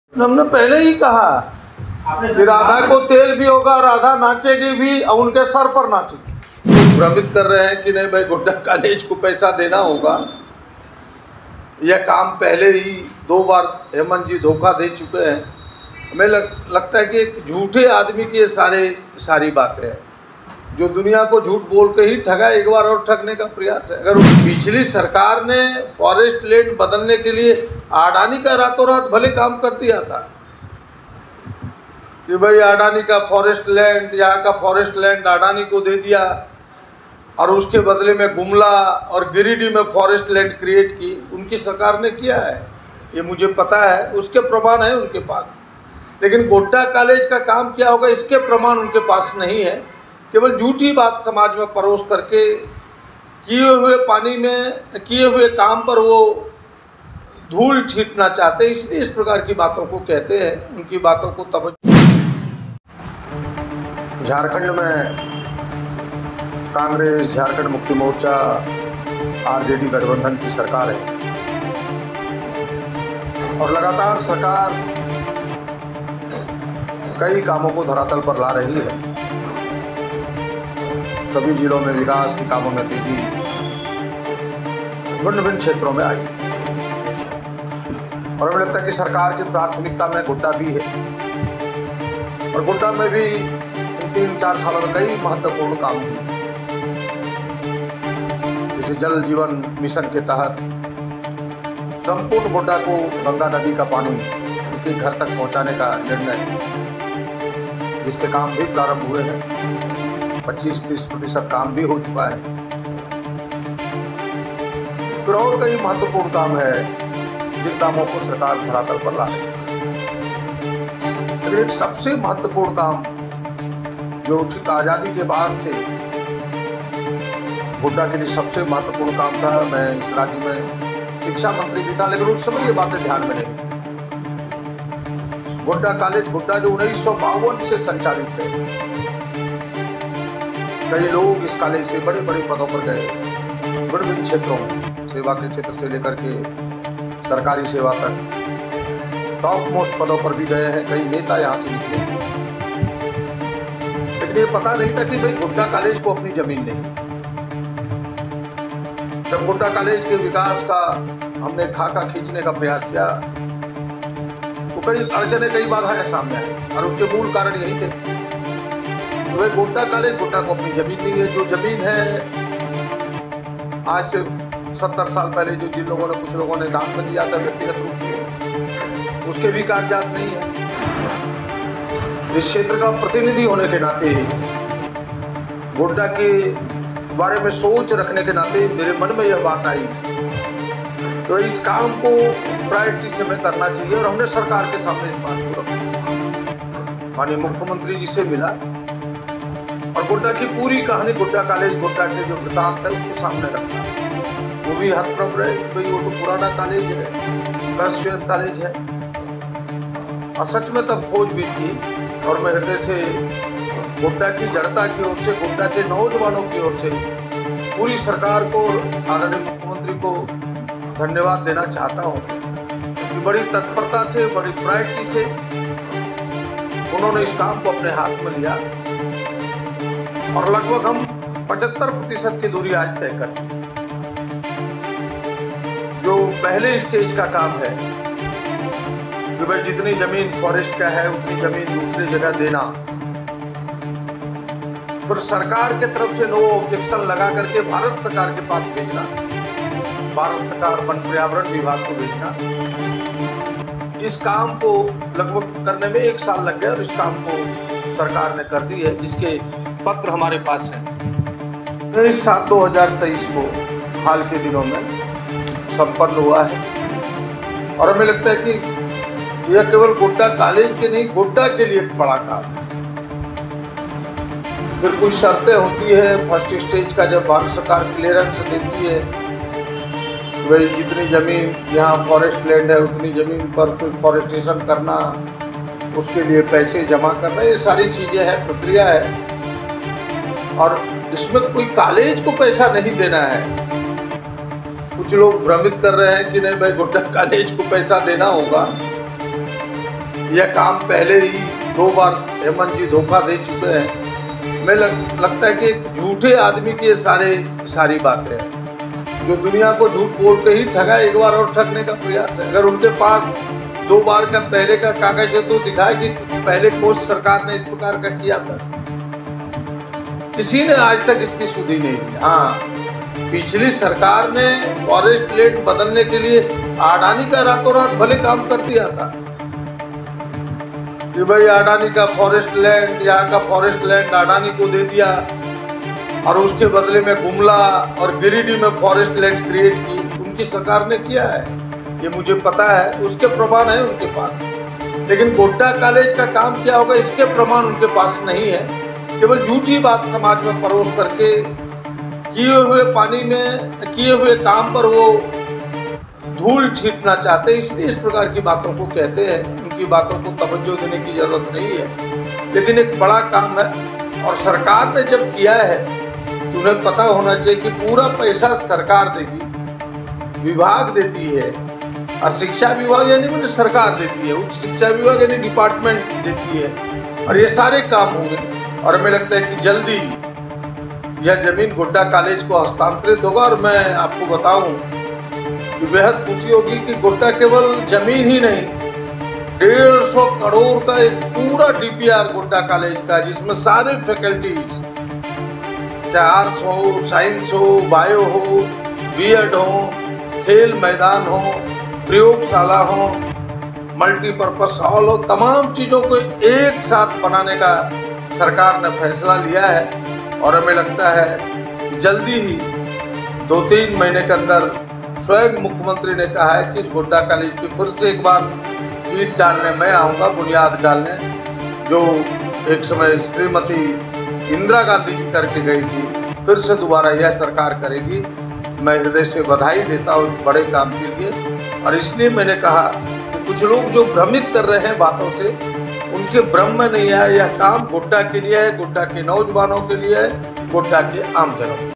सांसद निशिकांत के बयान पर विधायक प्रदीप यादव का पलटवार कहा राधा को तेल भी होगा और राधा नाचेगी भी!
सांसद निशिकांत के बयान पर विधायक प्रदीप यादव का पलटवार कहा राधा को तेल भी होगा और राधा नाचेगी भी और वो भी उनके सर पर । देखिए क्या है पूरा मामला !